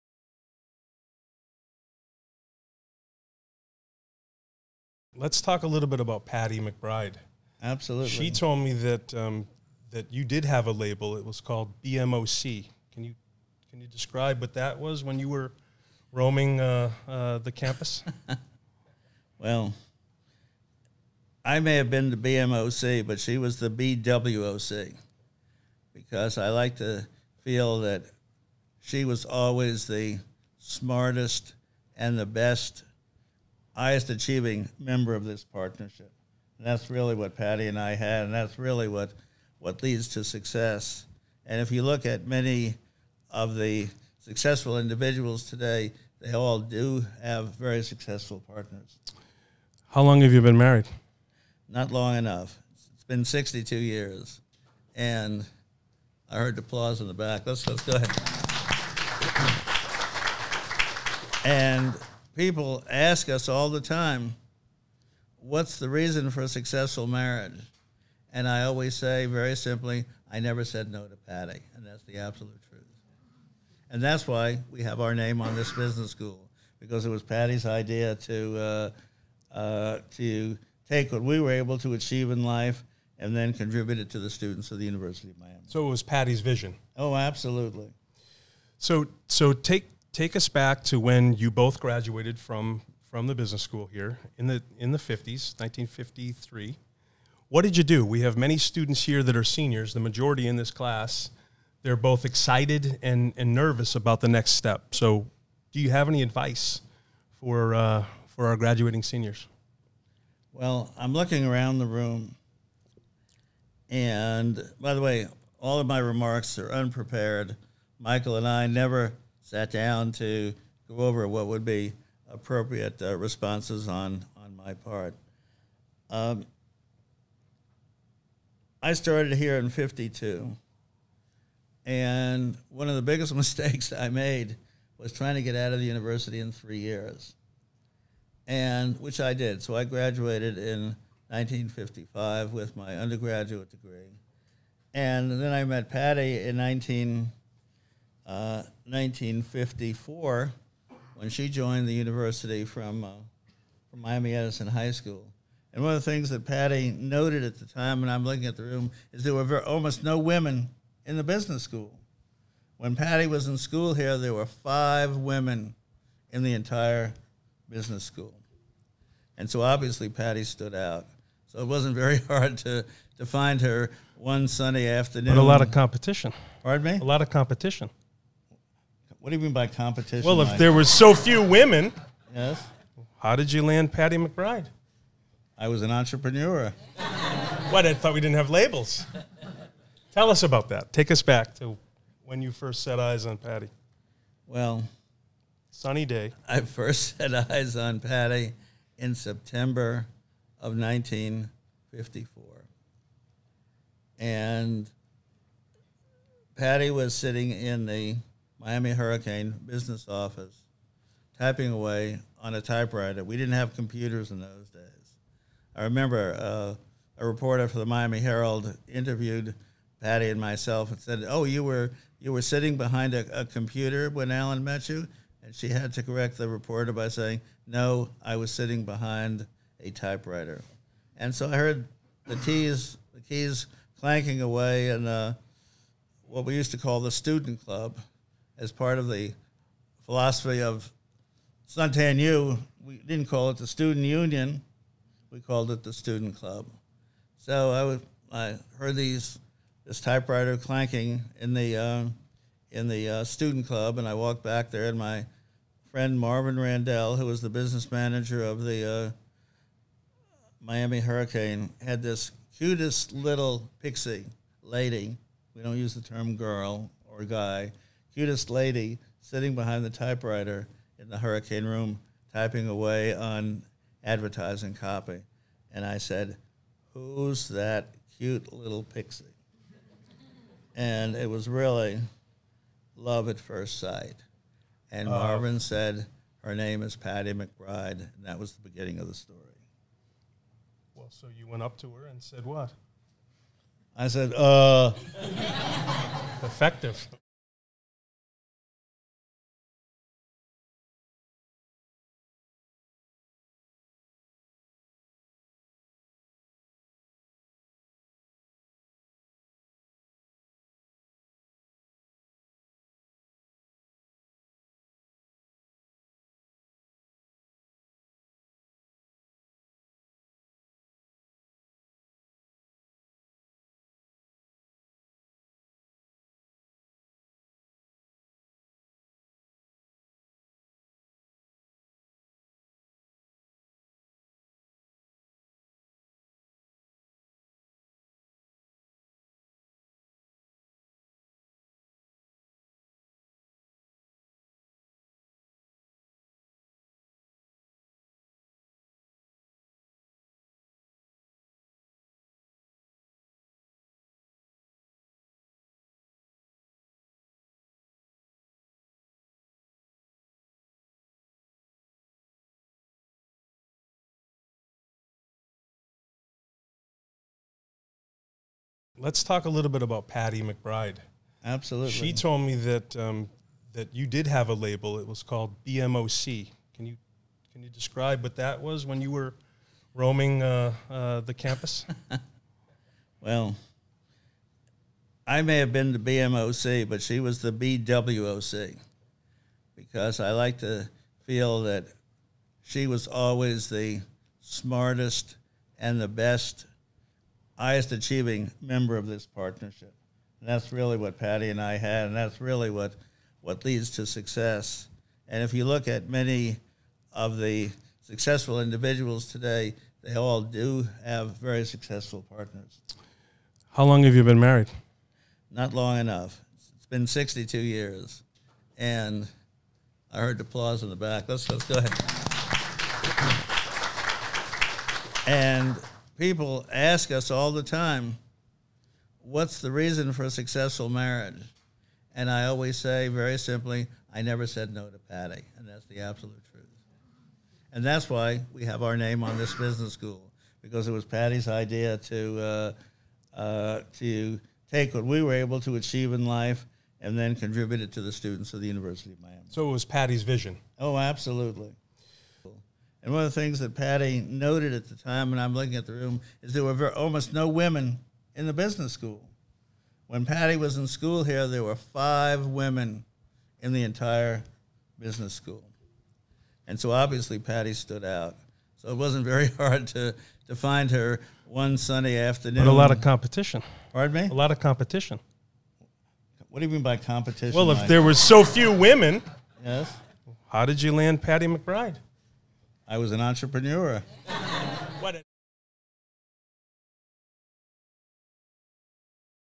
Allan Herbert On Finding The Love Of His Life At The University of Miami
Enjoy a clip of our full conversation with famed Miami businessman Allan Herbert. Miami Herbert Business School was recently renamed in honor of Allan and his wife Patti for their decades of work supporting the University of Miami community. Allan and Patti met as students at the Business School and have been married for more than 60 years. This clip is part of "Can We Teach It?" -- a multimedia effort that features conversations with path-breaking business leaders.